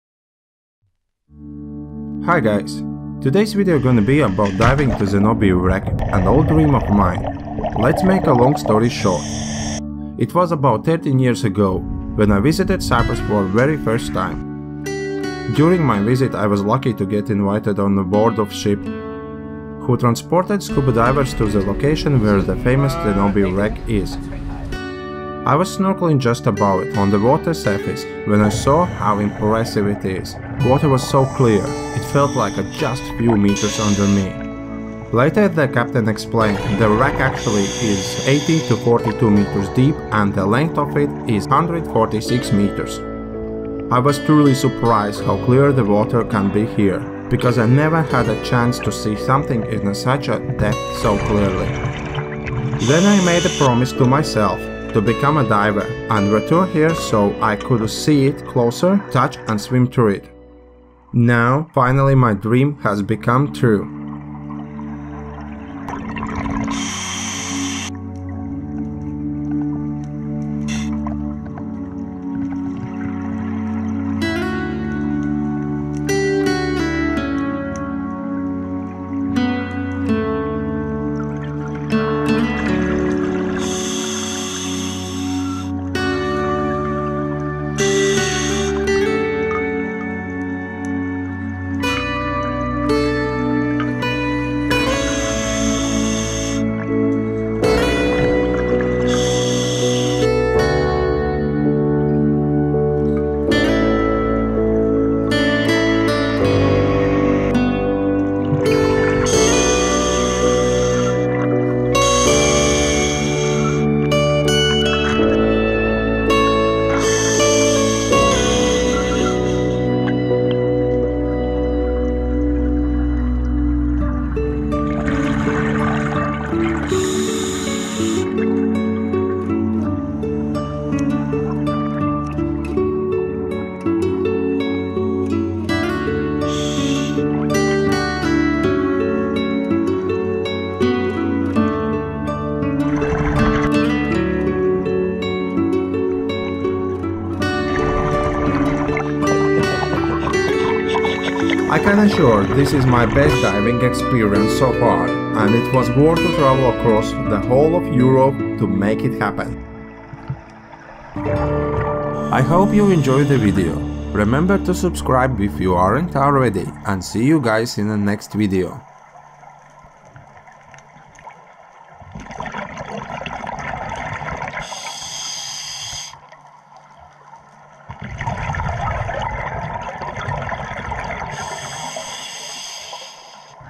Exploring Zenobia Wreck, Cyprus
Zenobia Wreck is one of the most magnificent shipwrecks in the world. Zenobia is resting at the bottom of the Mediterranean sea, at a  maximum depth of 42m and it's starboard side sits at 16m at it's shallowest. An unbroken wreck and its cargo look impressive! Wreck Zenobia is a must for any diver who is visiting Cyprus!